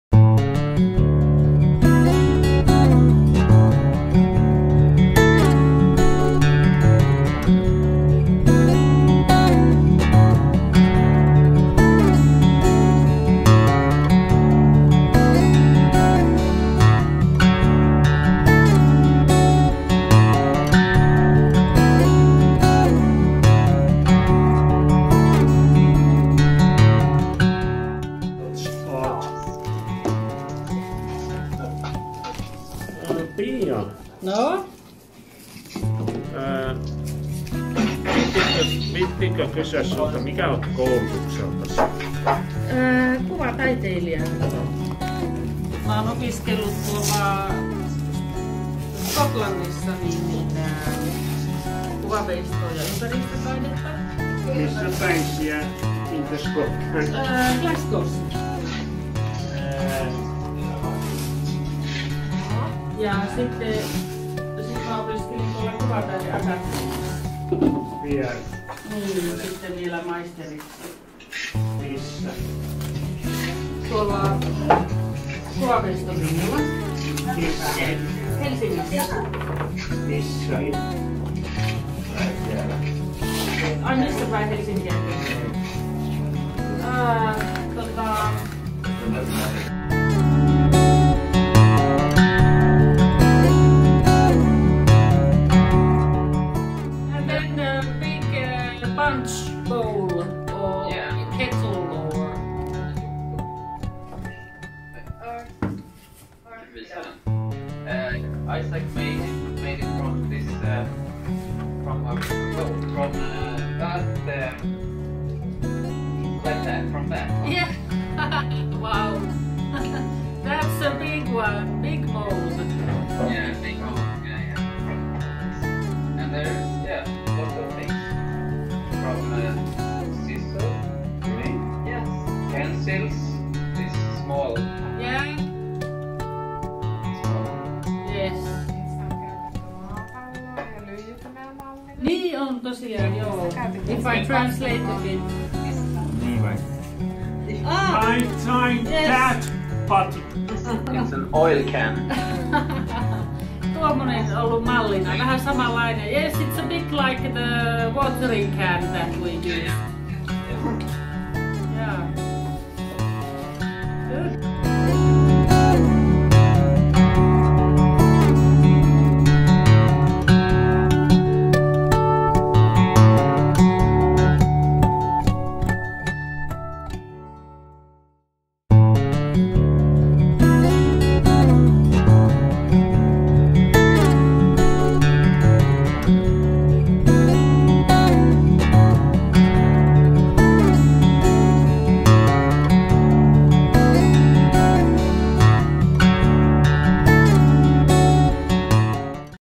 Kettuki's Online Workshop 23.8.2017
Online Workshop with participants from Autismisäätiö, Finland, and Konstskolan Linnea, Sweden, with mentor Pia Männikkö.